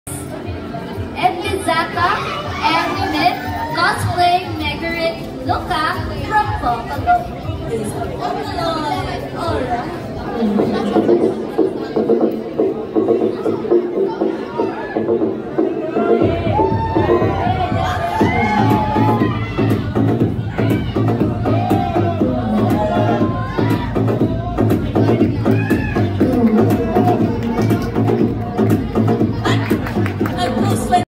Cosplay: Tanabata Festival - 20240818 150405
Fancam videos of the Tanabata Festival at Clark Cityfront Mall, Pampanga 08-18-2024